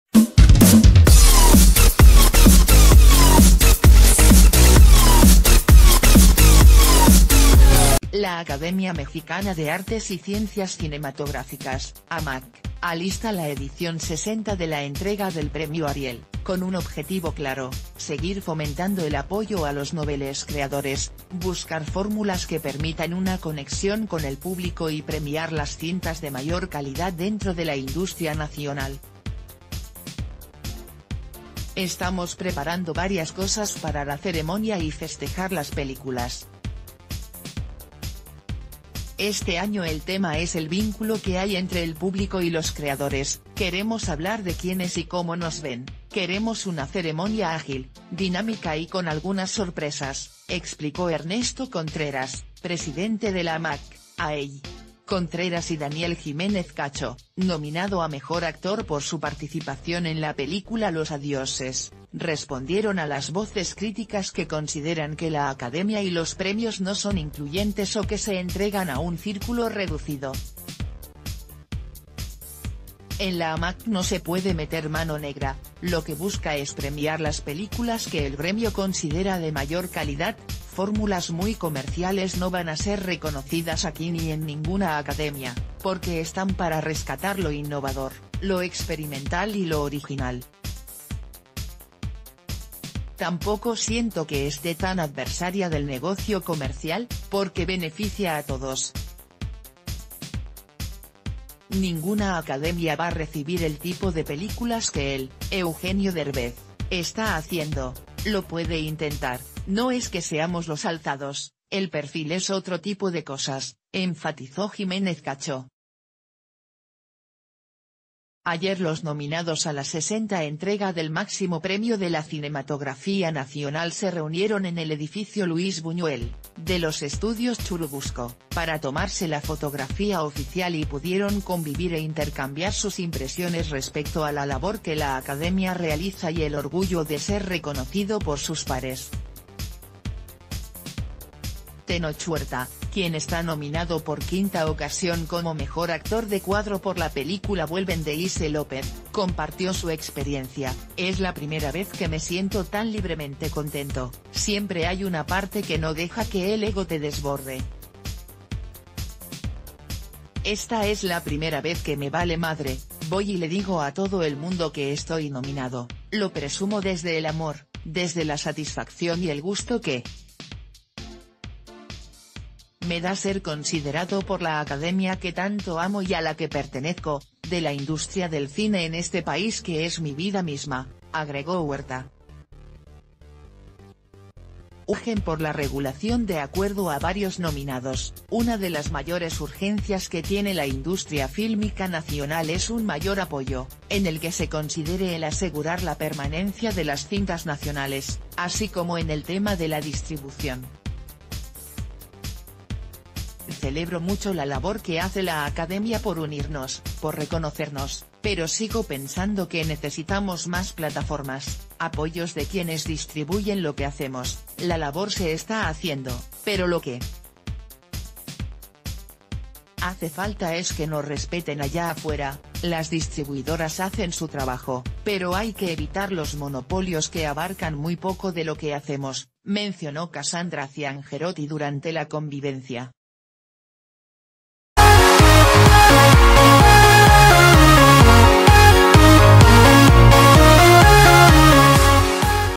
El Ariel apuesta porla innovación y la originalidad
La Academia Mexicana de Artes y Ciencias Cinematográficas (Amacc) alista la edición 60 de la entrega...